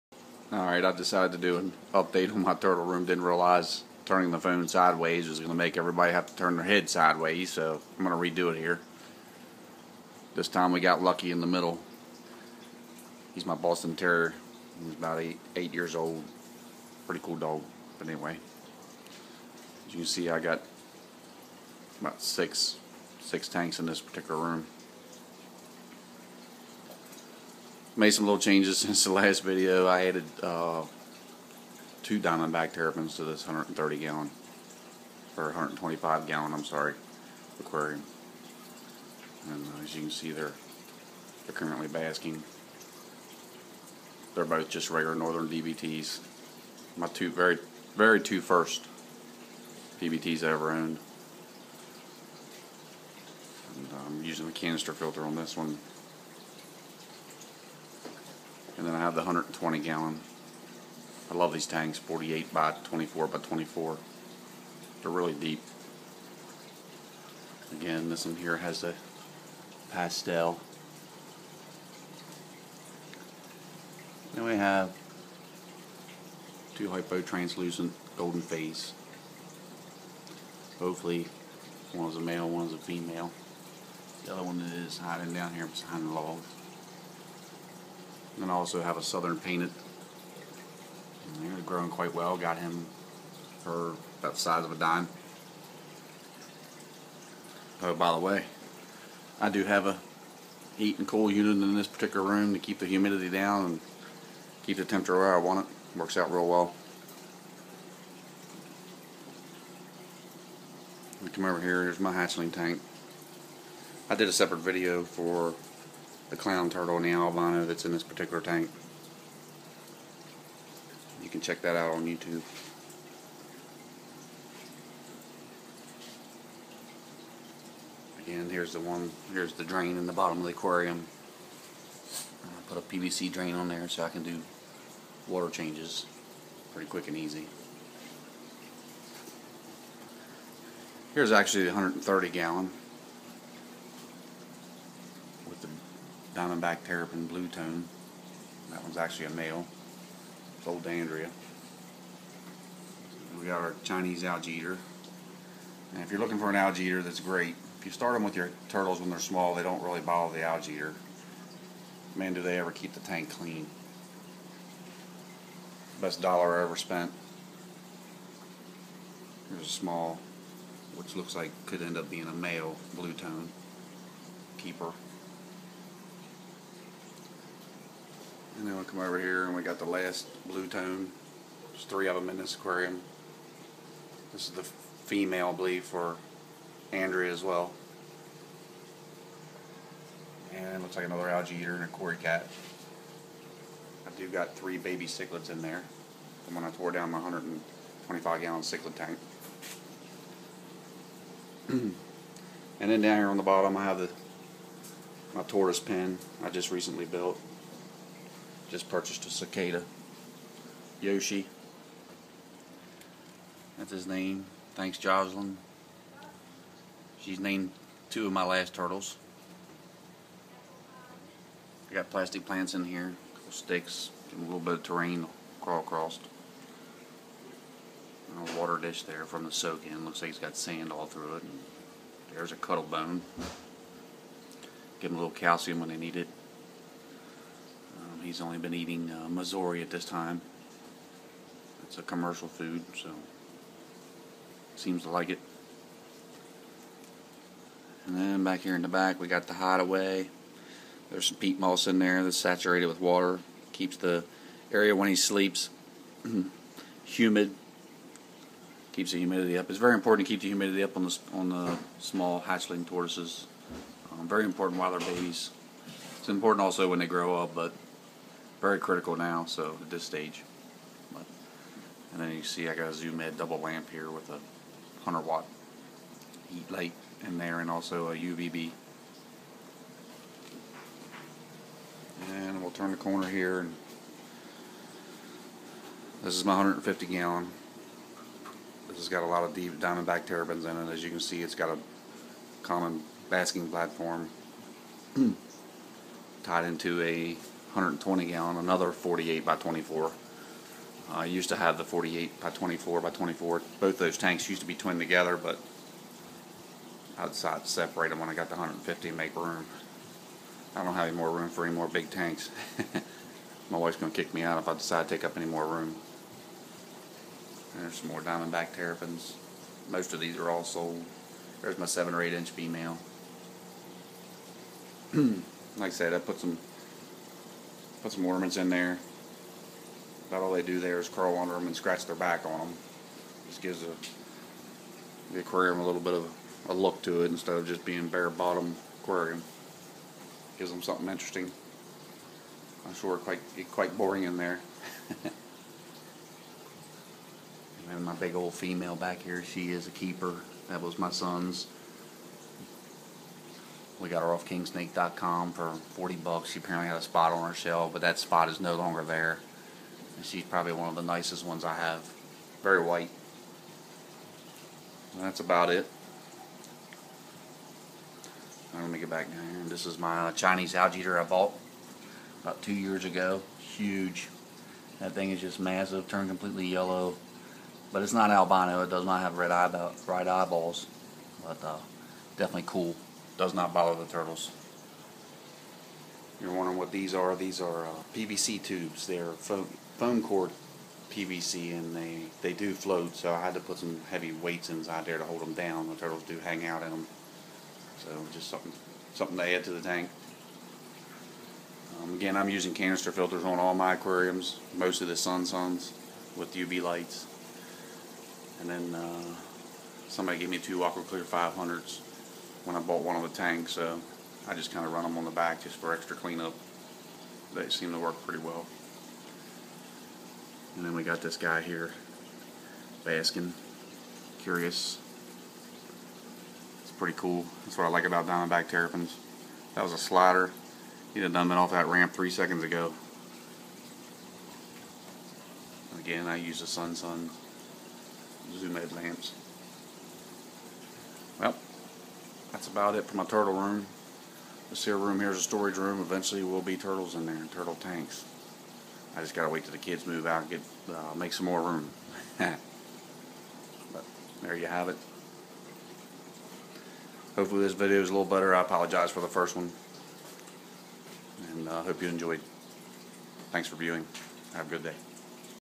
Turtles and Tortoise Room Update
6 aquariums total
1 hatchling tank
1 tortoise pen